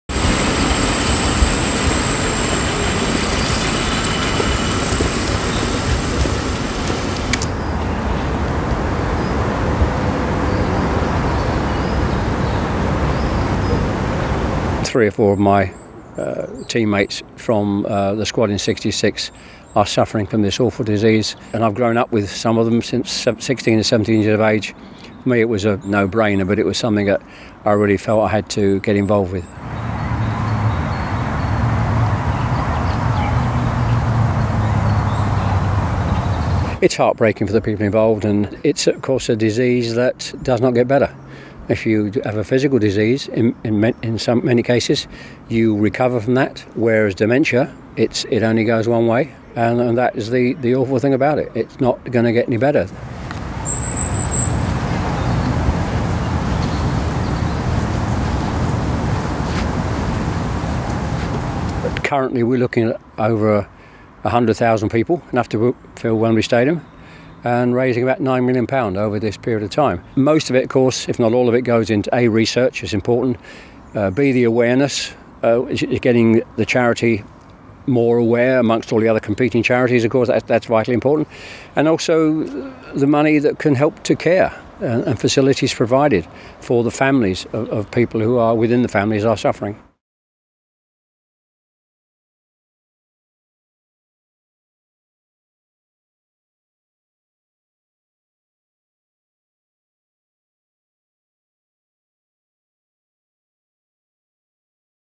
Sir Geoff Hurst On World Cup-Winning Team-Mates With Dementia
1966 World Cup winner Sir Geoff Hurst has joined the Alzheimer's Society Memory Walk campaign, which is aiming to help raise around £9 million to improve care, fund research and create lasting change for people affected by dementia.